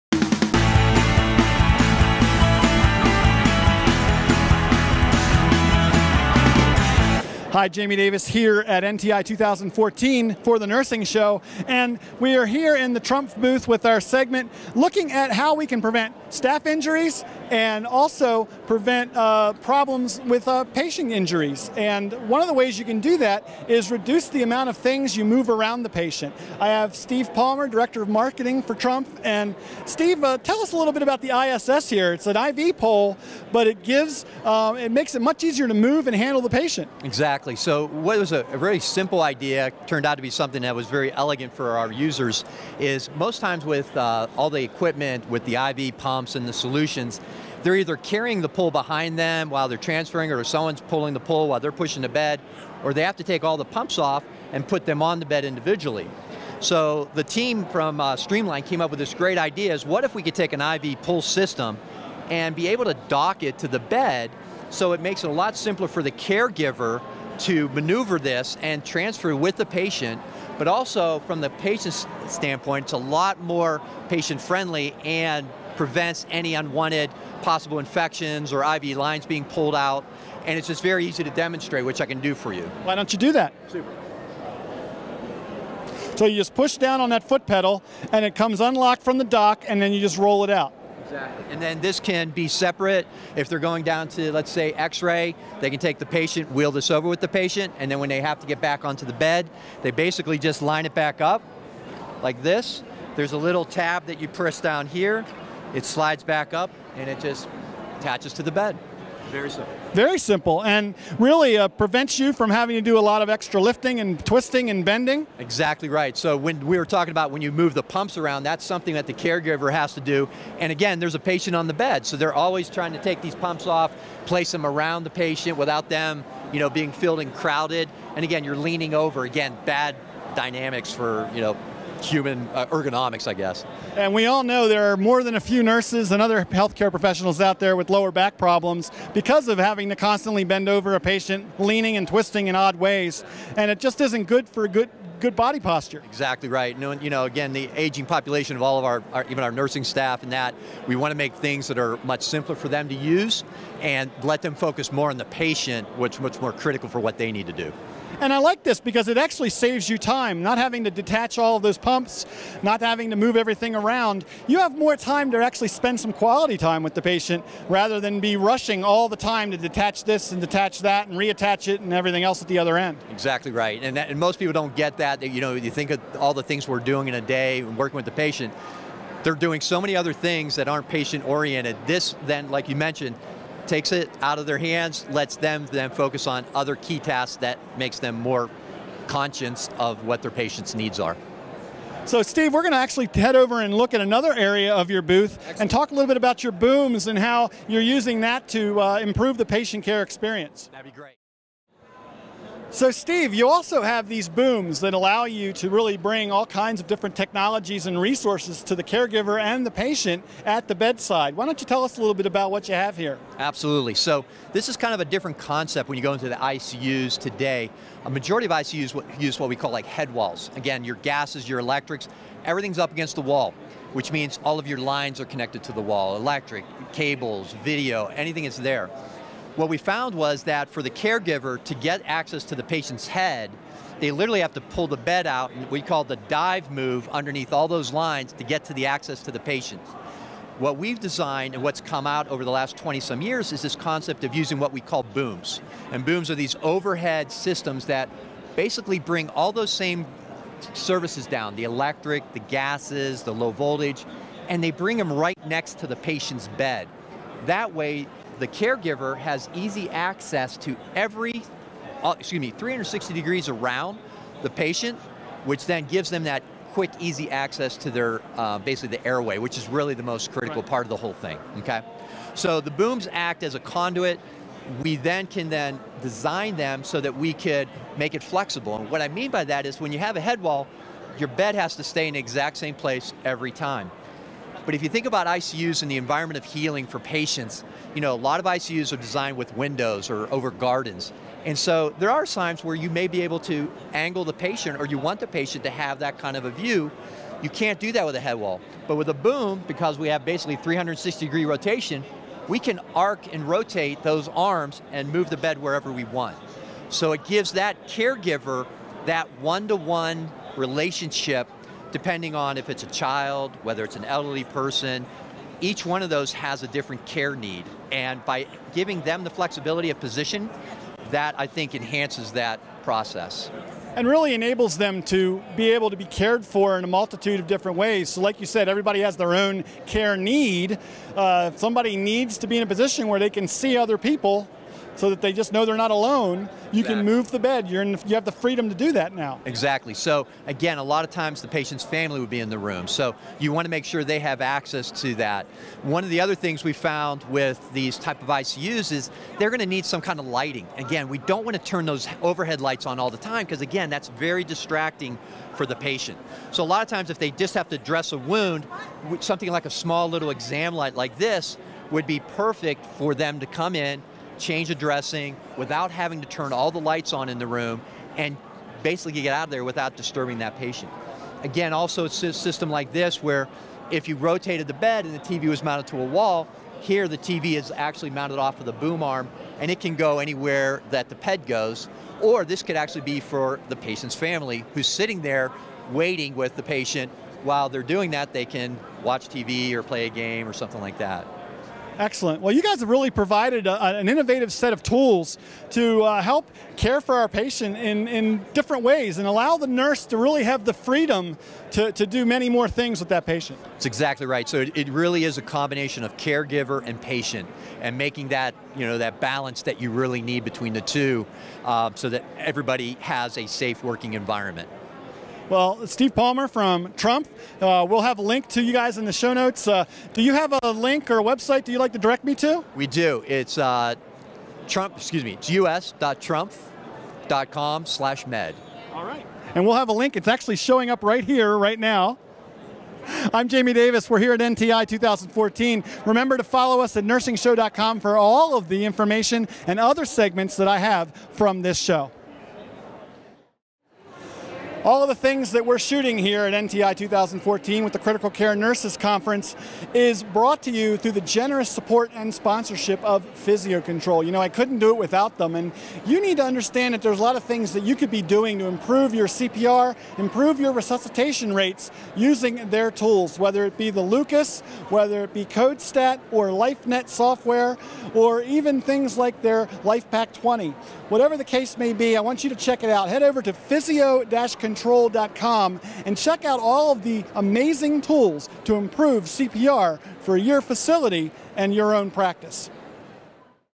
Trumpf Medical Systems Improve Patient Health with Innovative Tools for Bedside Nursing Care
Nursing Show host Jamie Davis visited the recent AACN critical care nurses' conference in Denver where he stopped by the Trumpf Medical booth to look at their new innovations in patient care tools for the bedside nurse.